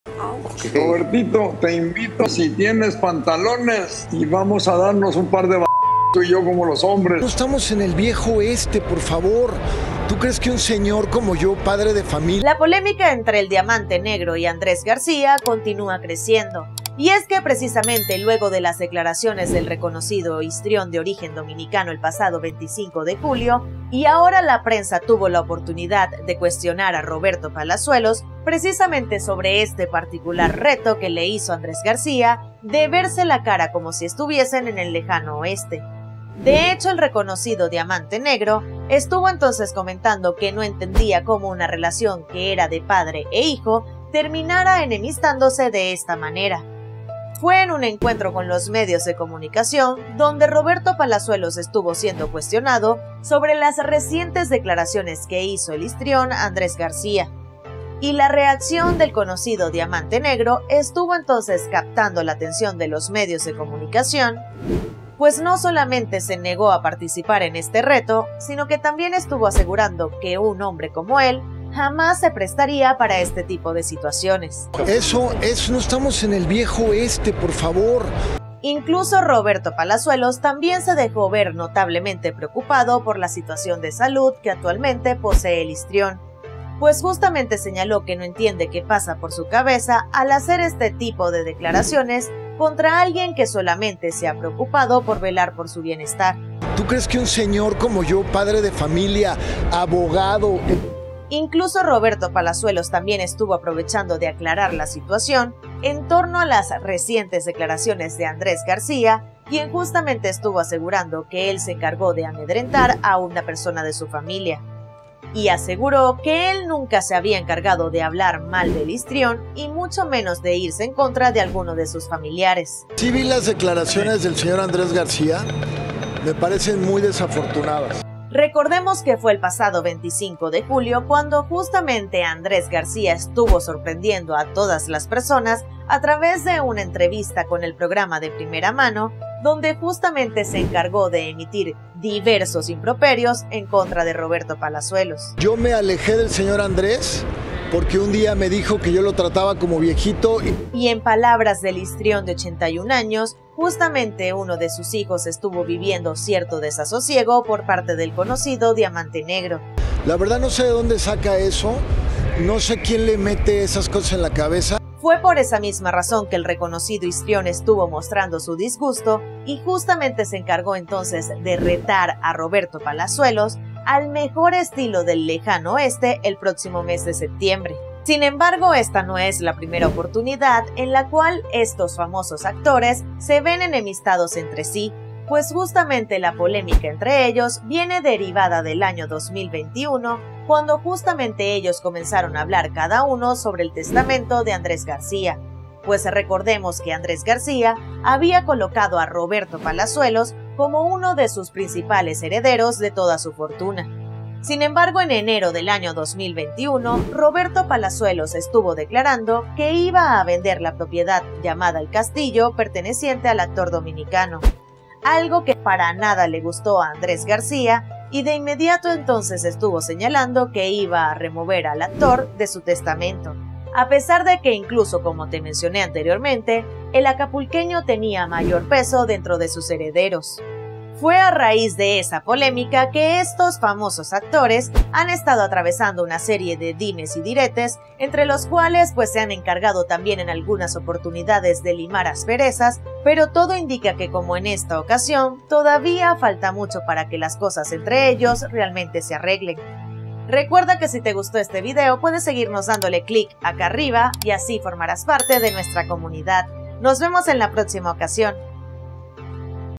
🛑😱 Roberto Palazuelos 𝗥𝗘𝗦𝗣𝗢𝗡𝗗𝗘 contundentemente a Andrés García
Con su temperamento de punta le envía este mensaje a Andrés…